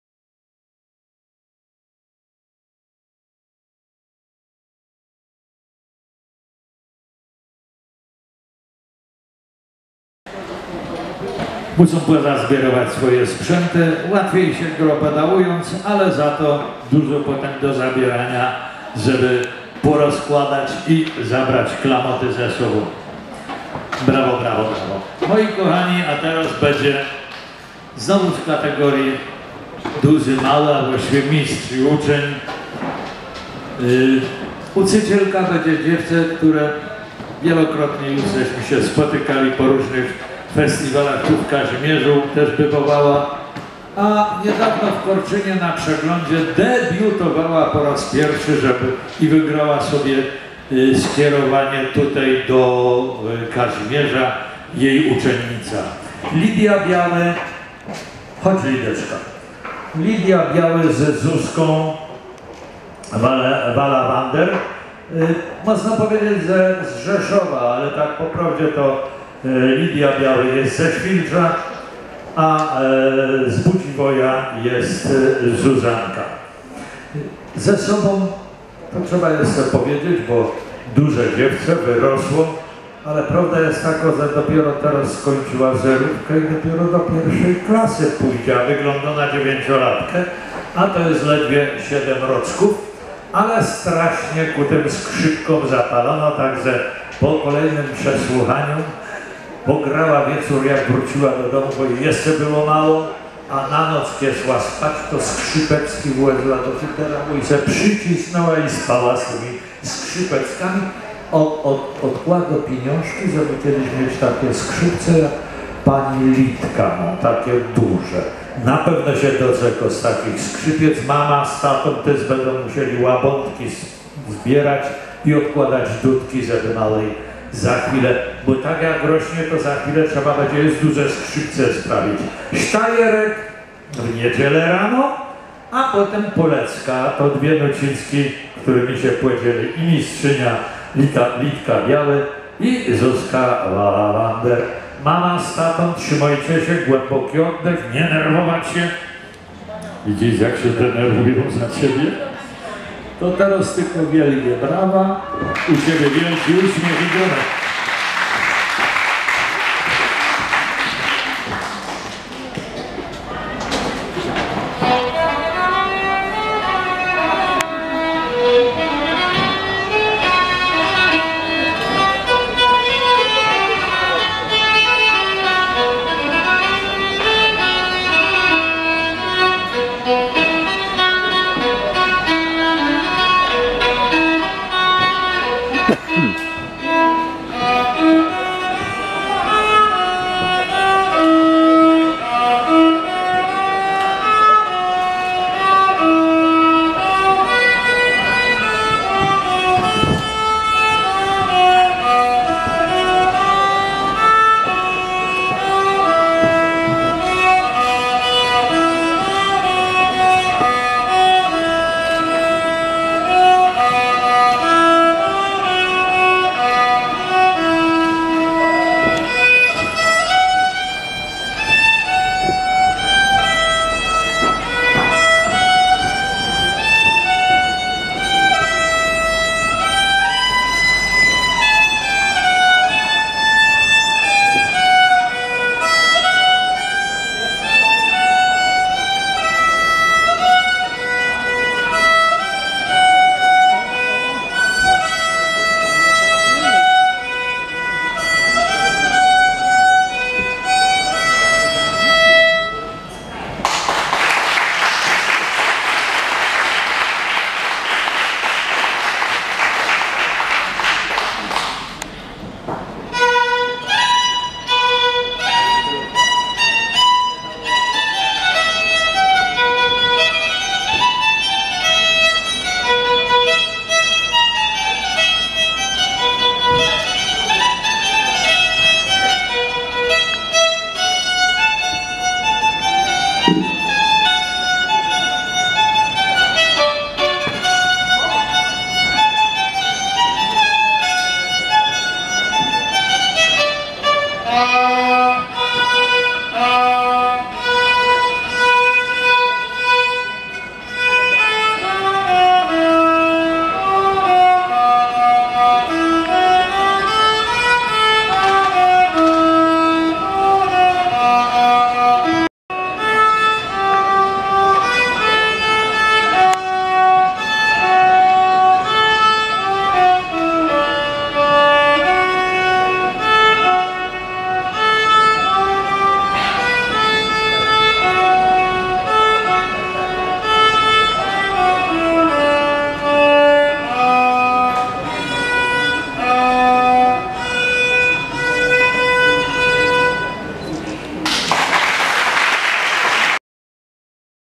STANEK49 LIDIA BIAŁY z ZUZANNĄ WALAWENDER/ RZESZÓW/KAZIMIERZ DOLNY-54 OFKiŚL-KAZIMIERZ D.29.08.2020
WYRÓŻNIENIE w KATEGORI MISTRZ I UCZEŃ w GRUPIE INSTRUMENTALISTÓW
54 OGÓLNOPOLSKI FESTIWAL KAPEL i ŚPIEWAKÓW KAZIMIERZ DOLNY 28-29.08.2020